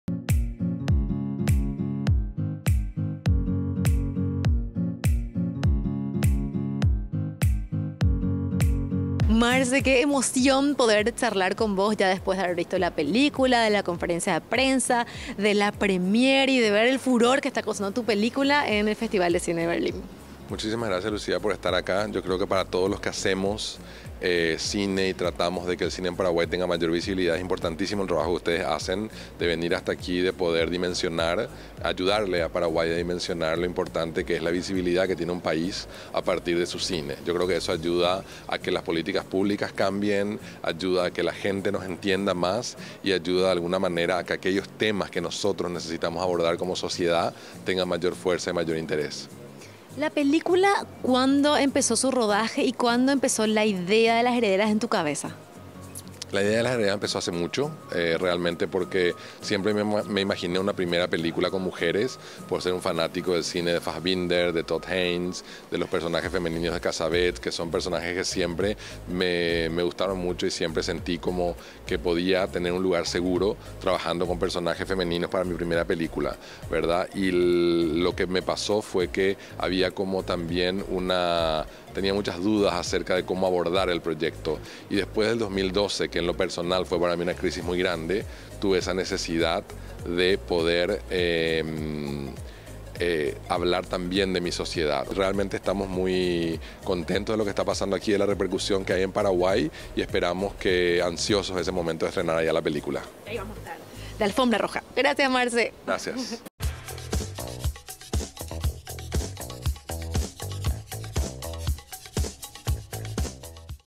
Las Herederas Entrevista a Marcelo Martinessi en la Berlinale 2018
En el marco de la edición nro 68 de la Berlinale Festival de cine de Berlín, conversamos con Marcelo Martinessi , director, guionista y co productor de la película que resultó ganadora de dos Osos de Plata . La entrevista se llevó a cabo en el Berlinale Palace dos días después de la Premiere y fue transmitida en la RPC Canal 13 Paraguay
Febrero 2018